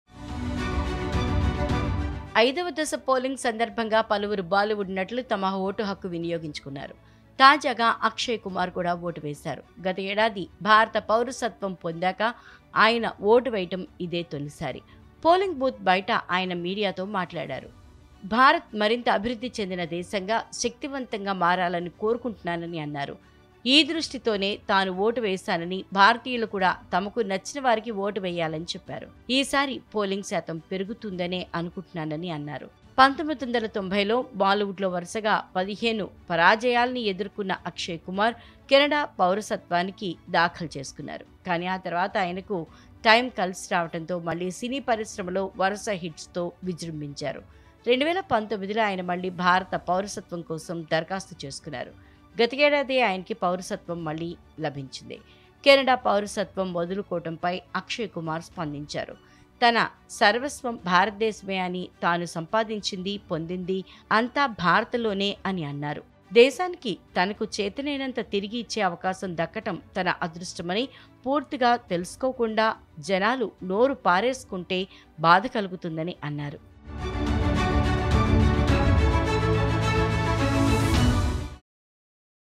1990ల్లో వరుసగా 15 పరాజయాలతో కెనడా పౌరసత్వం తీసుకున్న అక్షయ్ - TV9
ఐదవ దశ పోలింగ్‌ సందర్భంగా పలువురు బాలీవుడ్ నటులు తమ ఓటు హక్కు వినియోగించుకున్నారు. తాజాగా అక్షయ్ కుమార్ కూడా ఓటు వేశారు. గతేదాది భారత పౌరసత్వం పొందాక ఆయన ఓటు వేయడం ఇదే తొలిసారి. పోలింగ్ బూత్ బయట ఆయన మీడియాతో మాట్లాడారు.